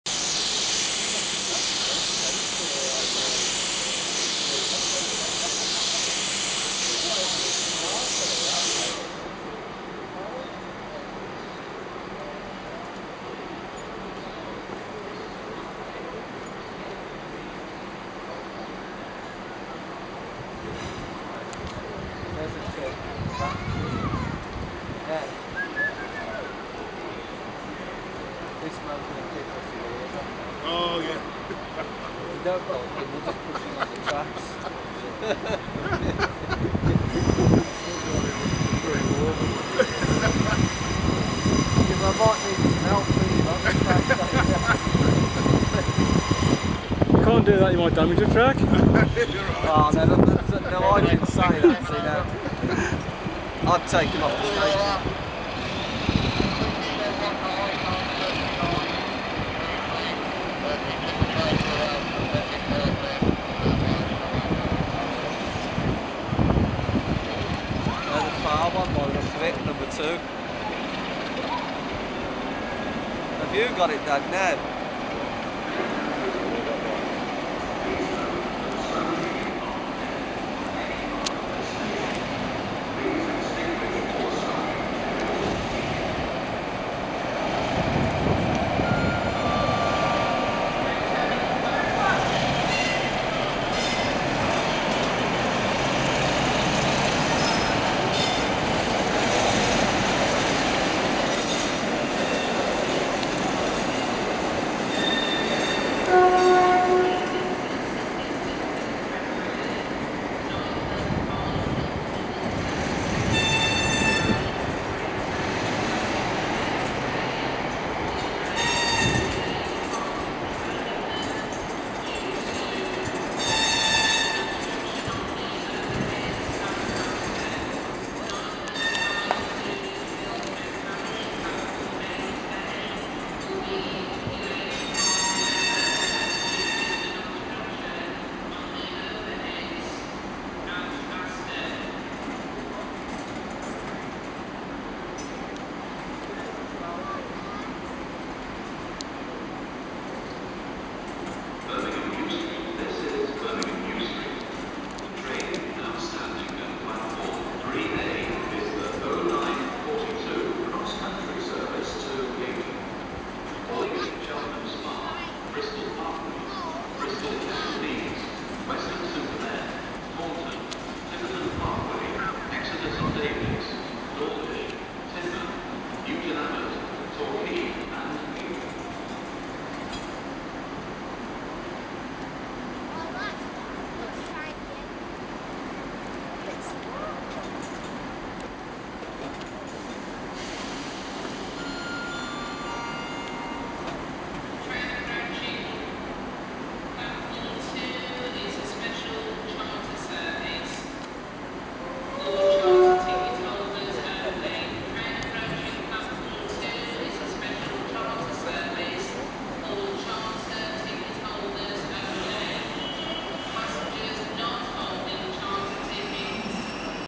5043 Earl of Mount Edgcumbe Arriving At Birmingham New Street Station 21-05-2011.avi
A video of 5043 Earl of Mount Edgcumbe entering New Street Station in Birmingham, with diesel D1755 / 47773 at the front on Saturday 21st May 2011